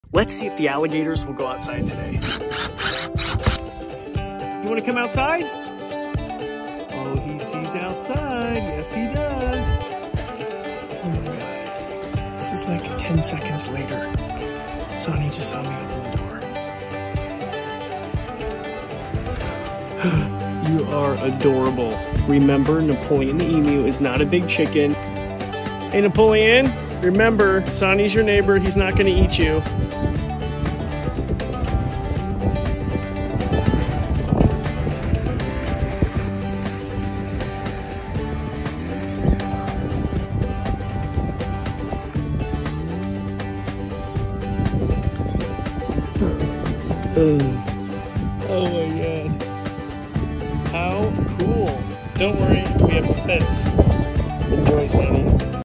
What Will Happen When I Open The Alligator Door??? 🤔🐊🚪
Watch as I open Soni the Alligator’s door outside!!!